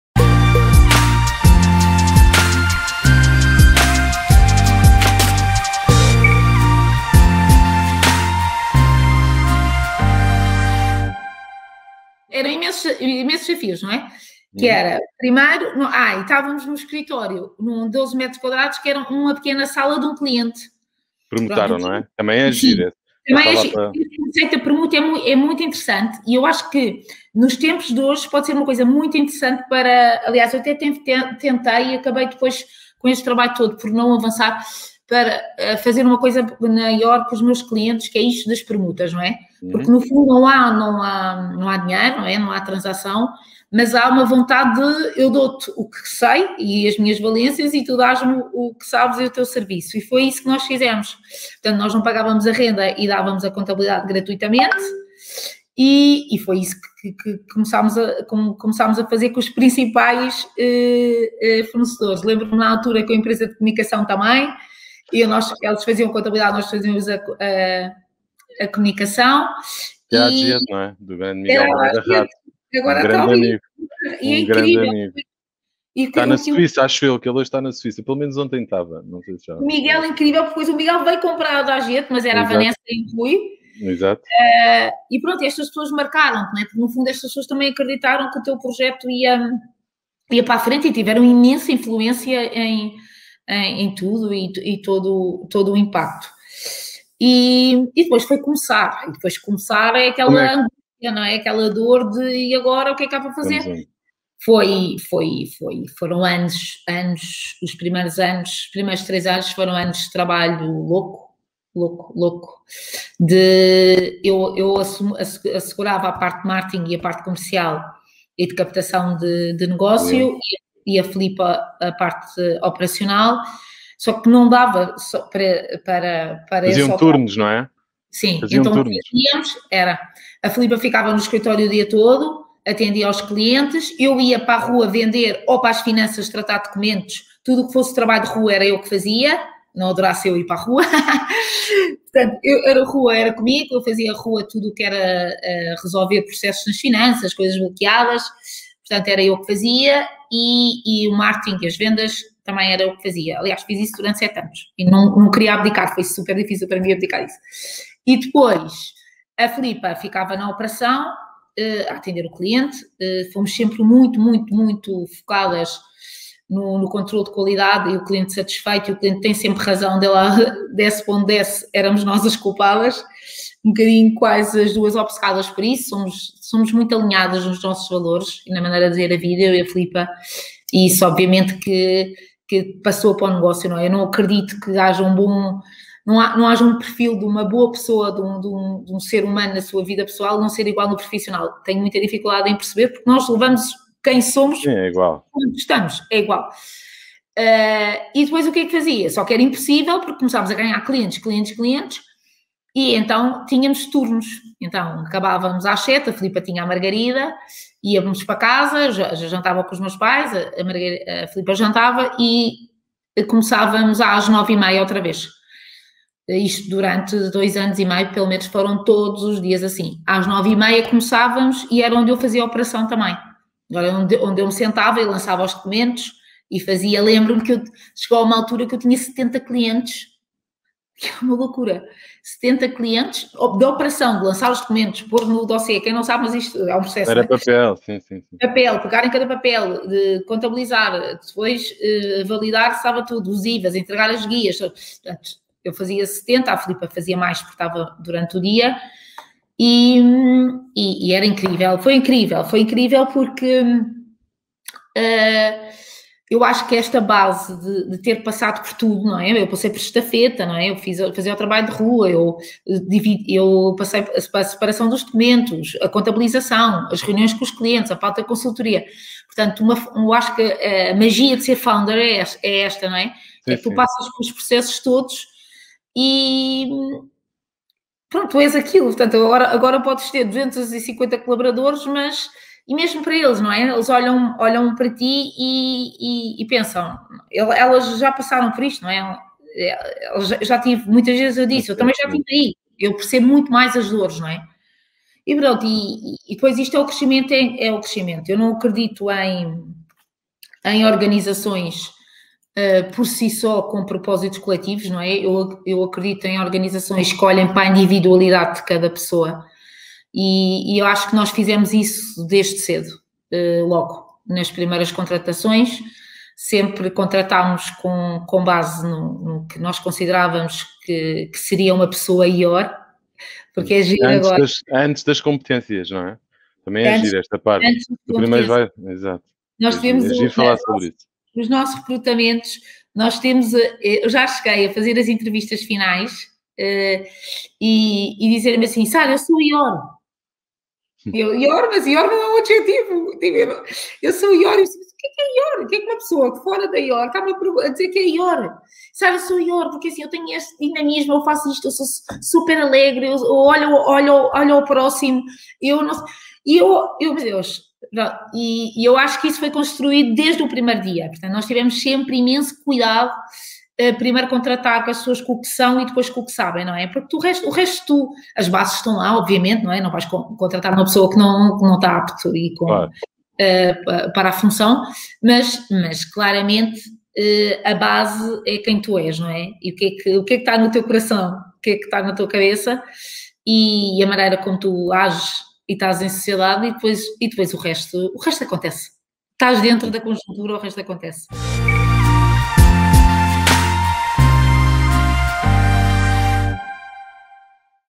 “A magia de ser founder é que passas pelos processos todos”
Sara do Ó, CEO do Grupo Your, fala sobre os desafios que enfrentou nos primeiros anos da empresa e admite que "tinha tudo para correr mal".